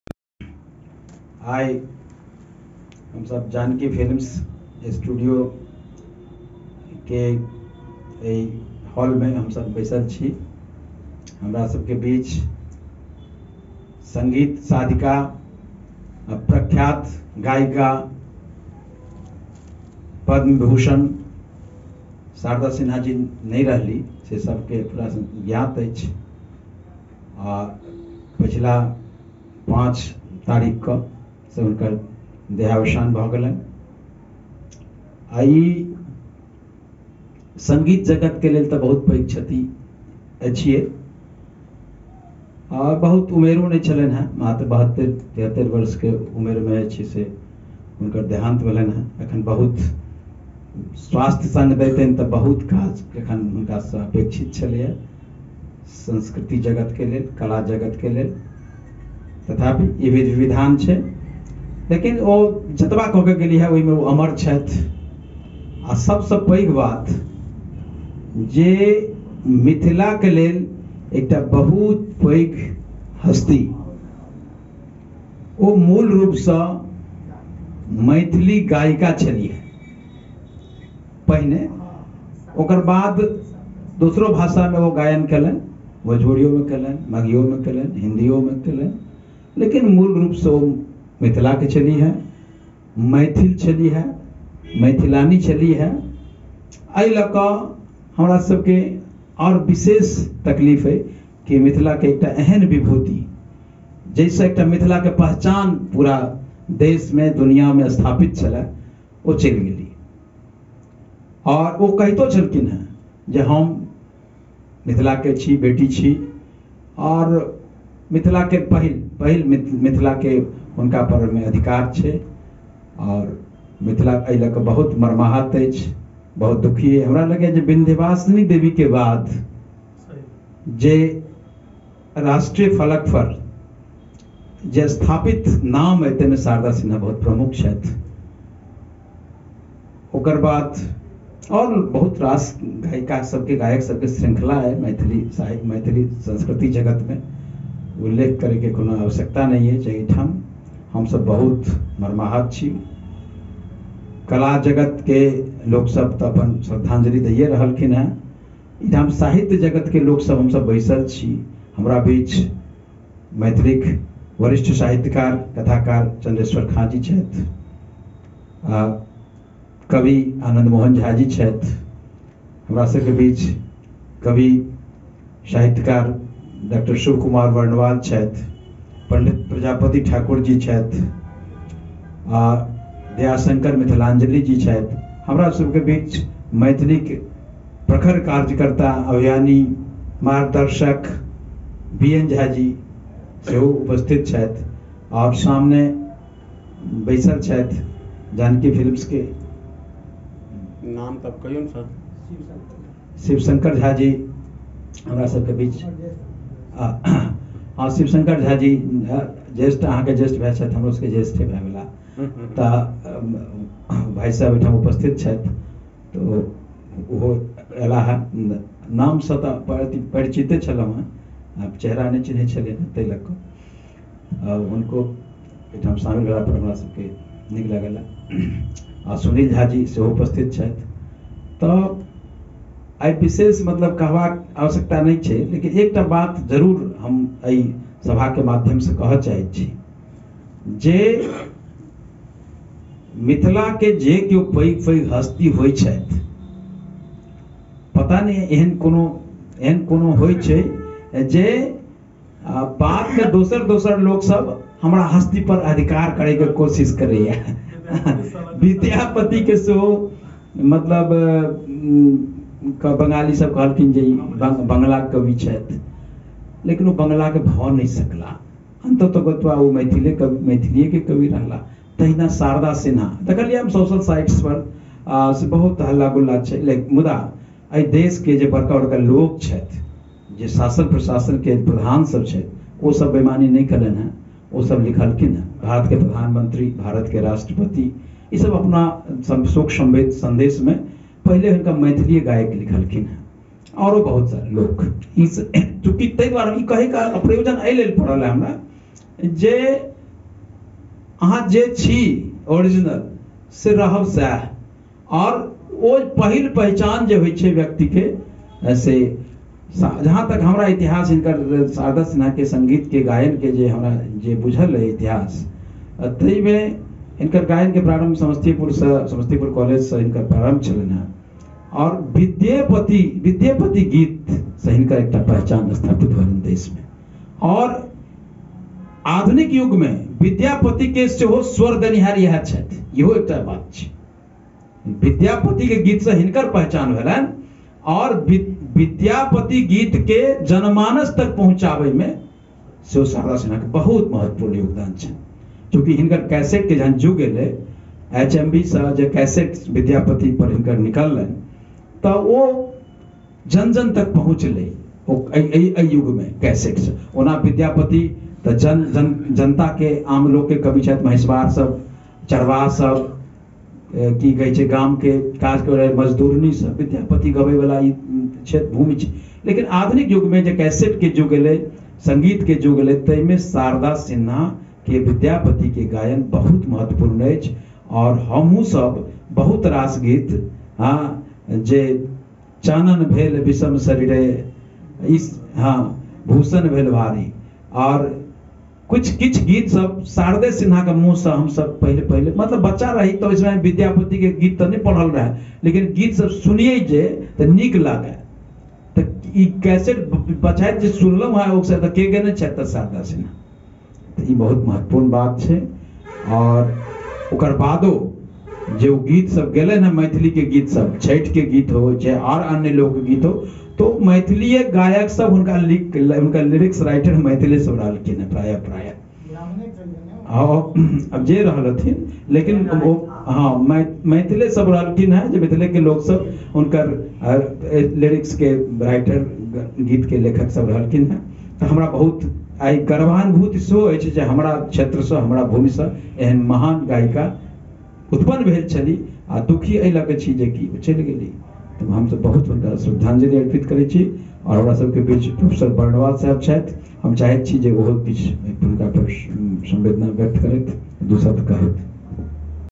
शारदा सिंहा जीक श्रधांजलि सभा में श्री दिलिप कुमार झा
जानकी फिल्म प्रोडक्शन्स प्राइवेट लिमिटेड कंपनी मधुबनी मे पदम् श्री श्रीमती शारदा सिंहा जी  के श्रधांजलि सभा